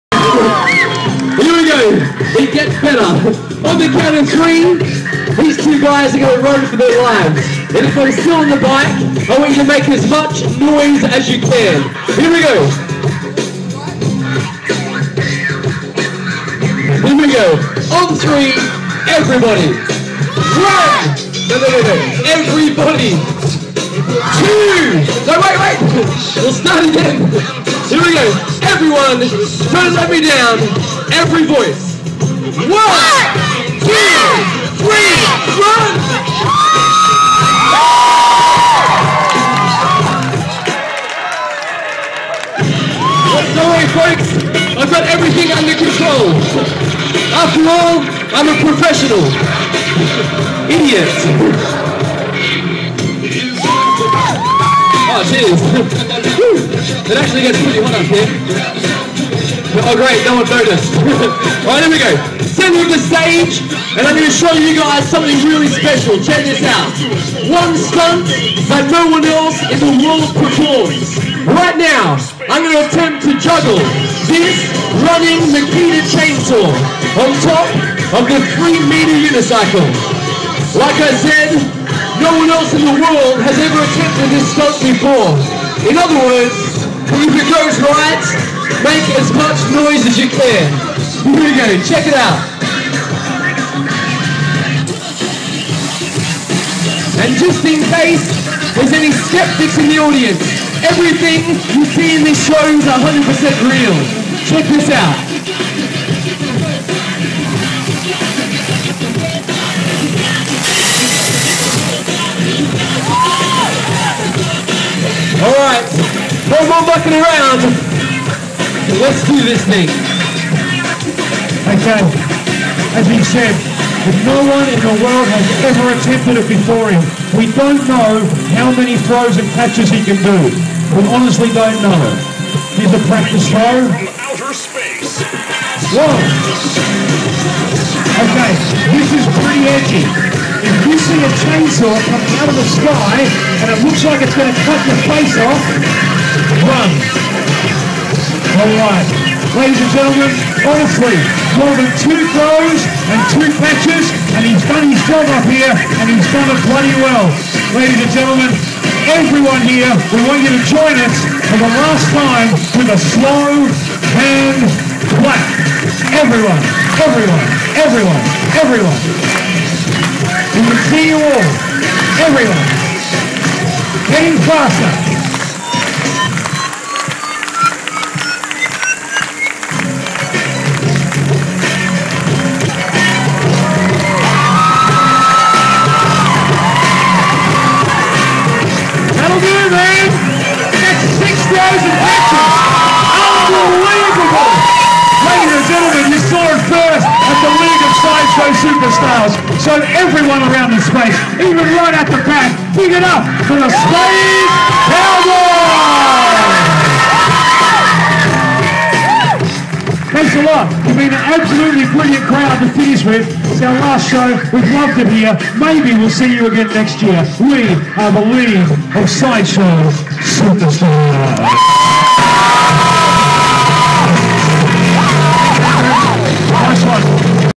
Chainsaw juggling on tall unicycle!
The Space Cowboy (AKA: Chayne Hultgren) juggling a running chainsaw while riding a 3 meter unicycle.
The first and only person to perform this deadly stunt!
Adelaide Royal show. Sept 2010